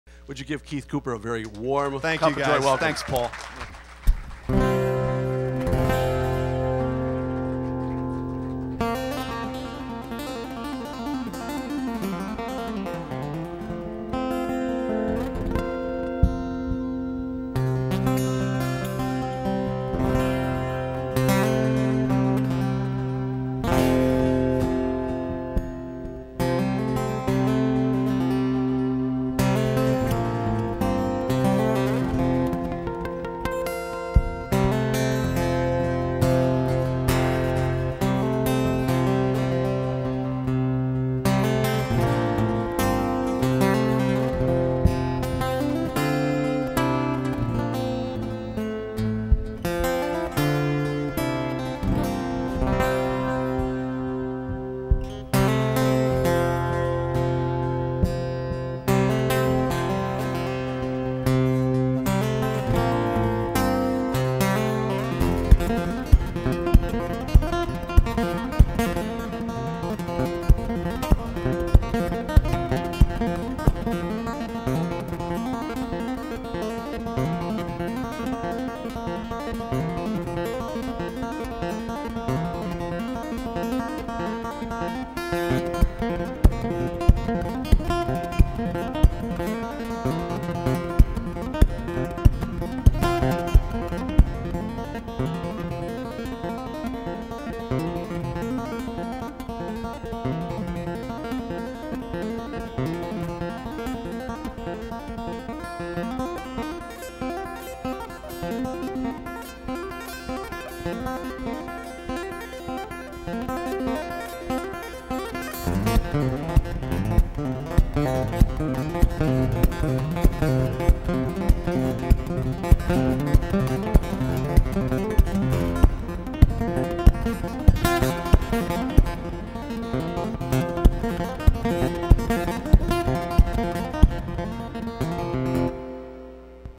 Keith Cooper 2013
Keith Cooper performing at the Cup O Joy